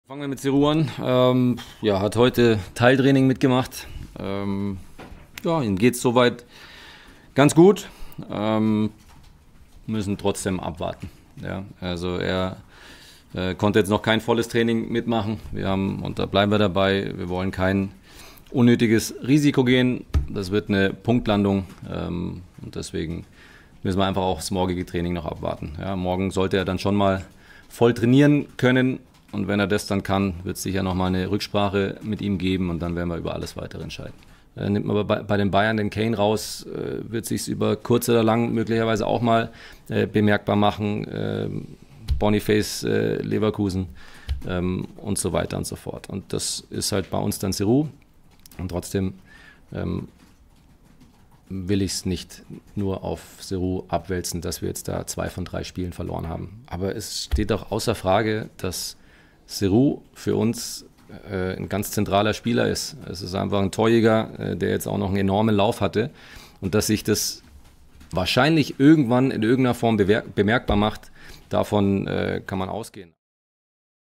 Guirassy-Einsatz gegen BVB? Hoeneß hofft auf "Punktlandung" | SID
Der VfB Stuttgart muss wohl bis zur letzten Minute um einen Einsatz von Toptorjäger Serhou Guirassy bangen. Trainer Sebastian Hoeneß hofft auf eine "Punktlandung" zum Spiel gegen Borussia Dortmund. Guirassy war mit 14 Toren in den ersten acht Bundesliga-Spielen der Garant für den starken Saisonauftakt der Stuttgarter. Aktuell zeigt die Formkurve ohne den Stürmer nach unten.